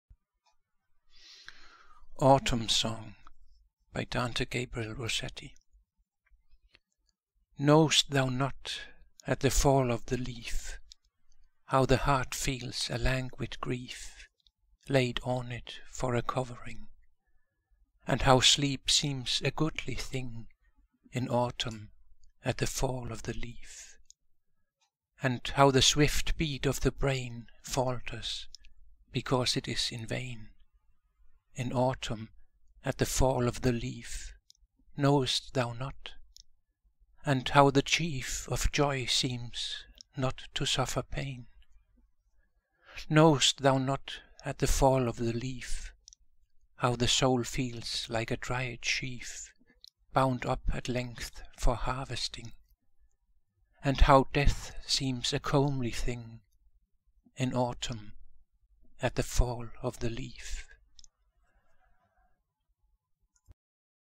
"Autumn Song" by Dante Gabriel Rossetti, recited by The Wordman
The poems of Dante Rossetti are so genuine, that they break your heart, whether they are sad or happy - they are all BRILLIANT! Like this ...